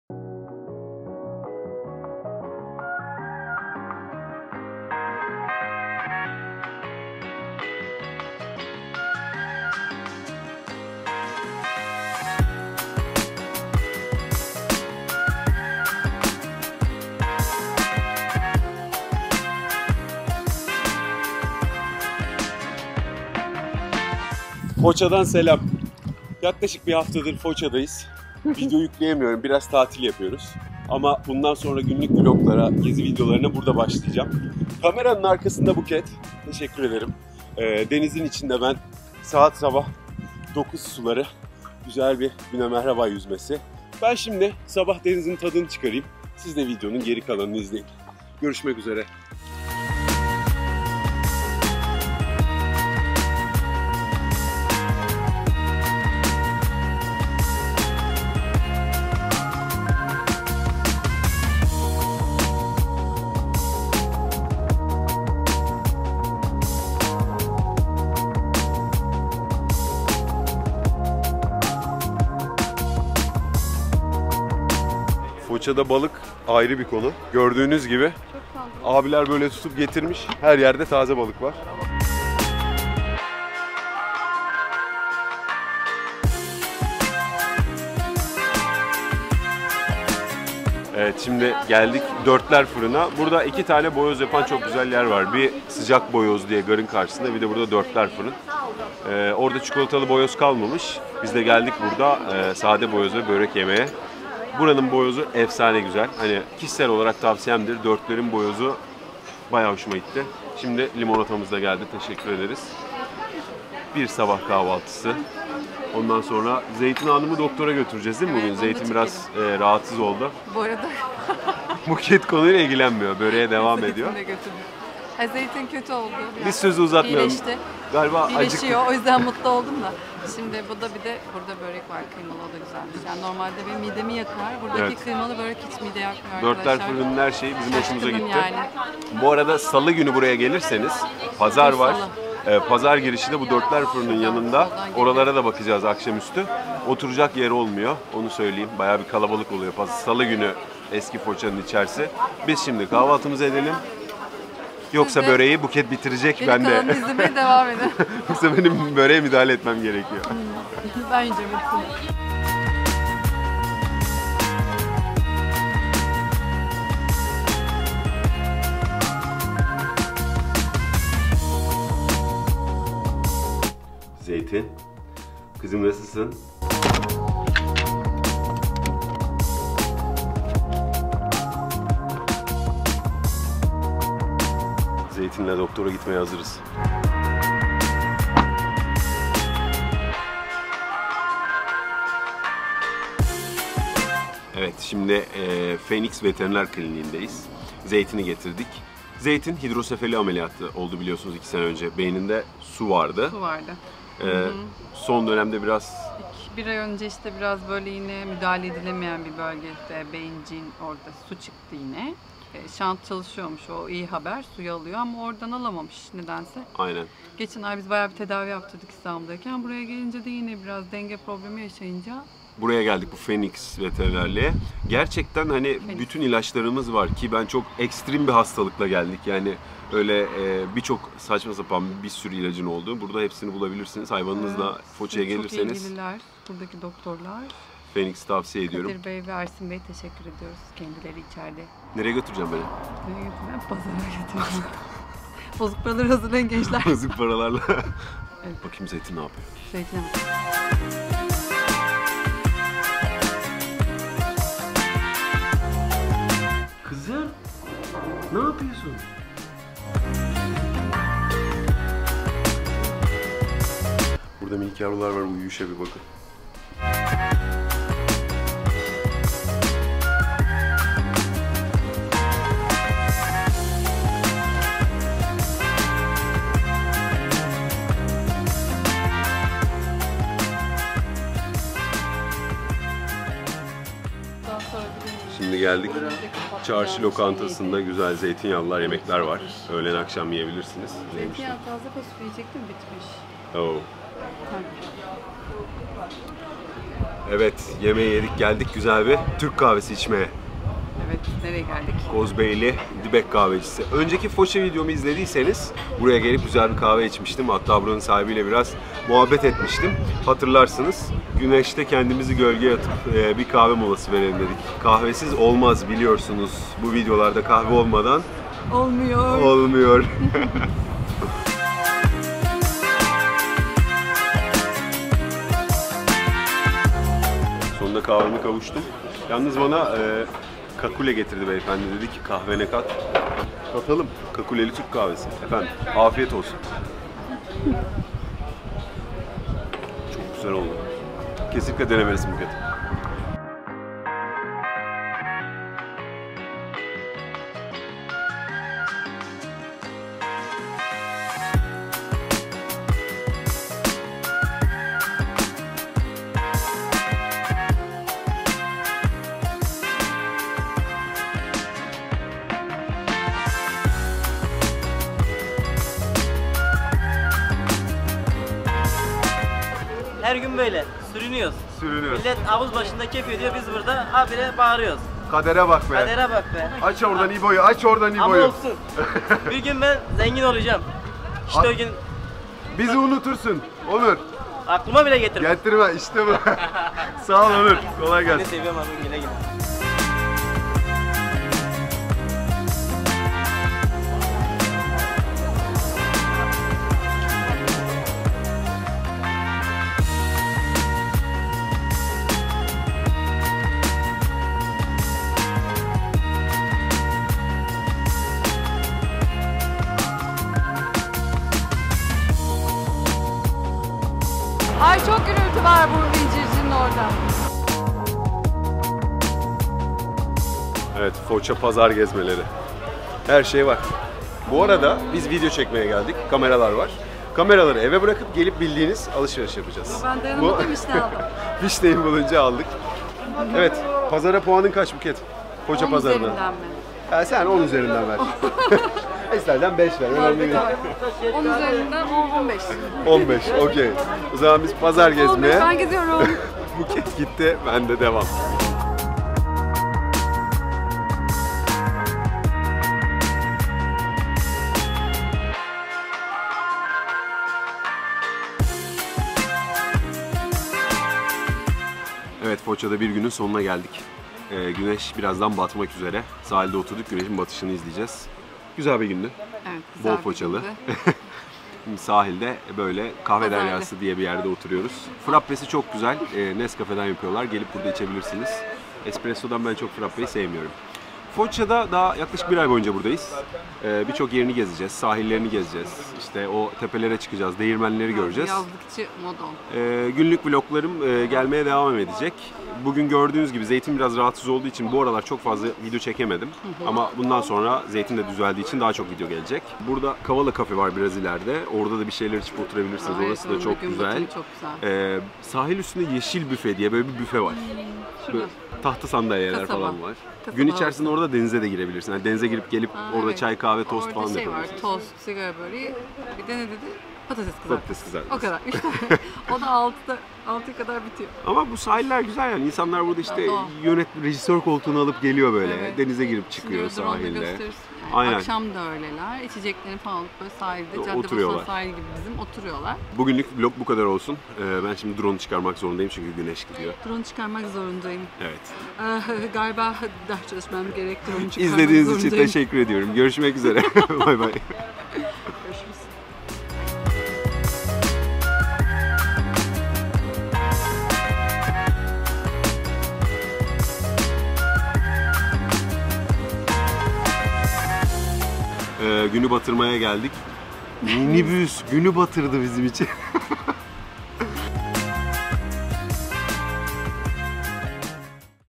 ESKİ FOÇA'DA SIRADAN OLMAYAN BİR GÜN
Uzunca bir süredir çok fazla vlog çekemedim. (Sebebi bu vlogda diyerek merak uyandırayım 😉) Tatilimi geçirdiği Eski Foça'da günlük bir vlog ile karşınızdayım. Foça'da bir günüm.